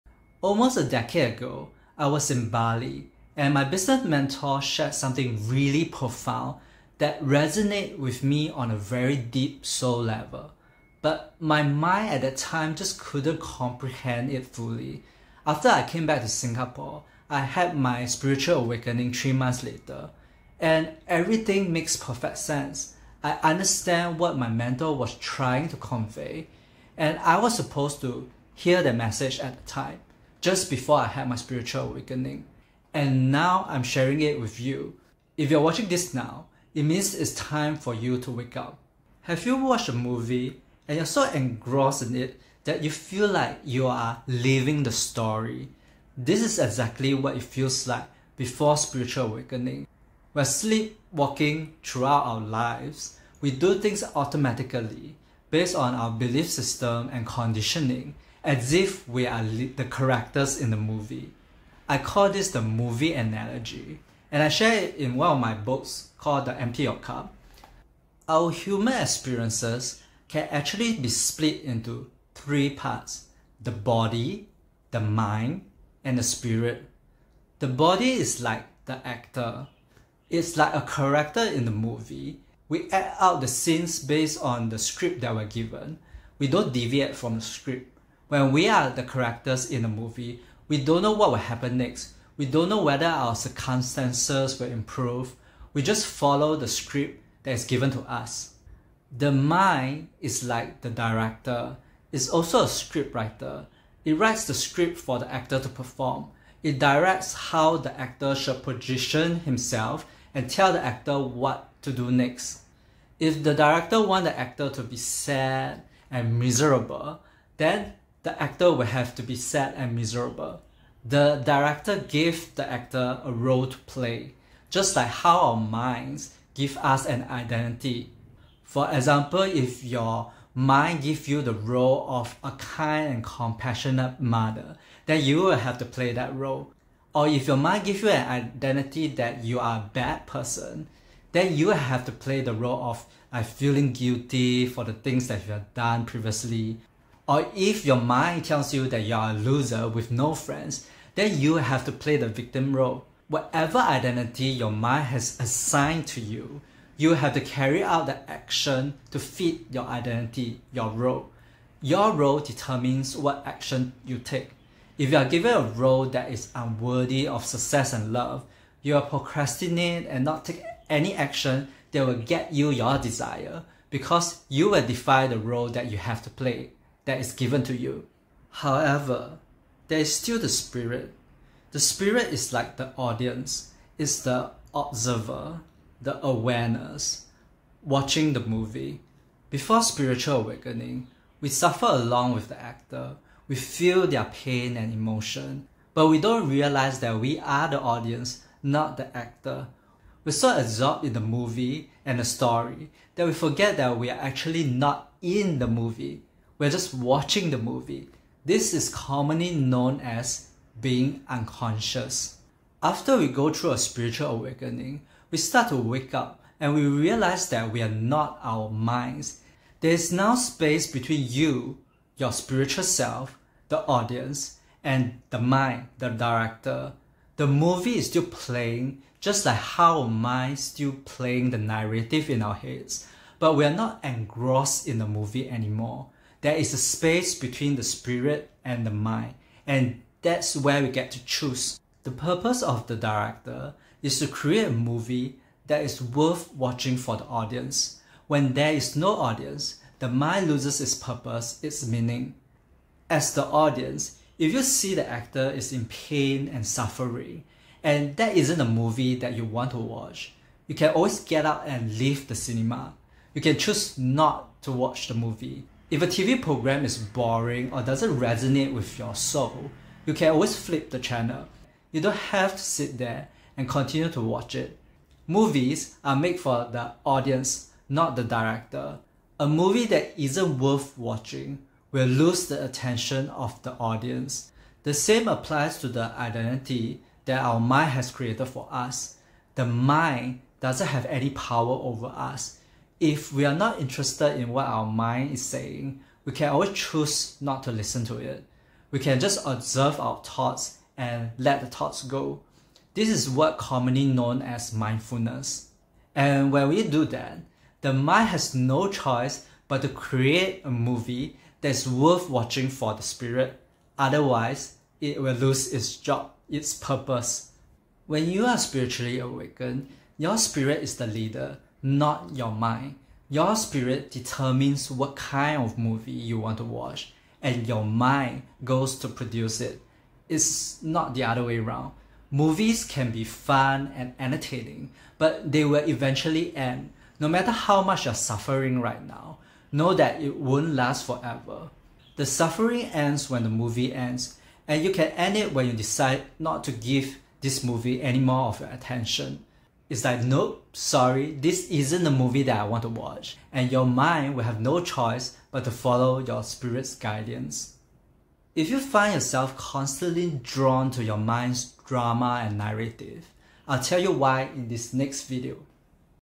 If You’re Seeing This, It’s Time to Wake Up
Before I was spiritually awakened, my mentor shared this profound message with me.

In this video, I'll pass this insight to you. If you are seeing this, it means you are ready for spiritual awakening. This is the message the Universe wants you to hear.

Using the analogy of watching a movie, I will explain what spiritual awakening is and help you understand the difference between your body, mind, and spirit.

Empty Your Cup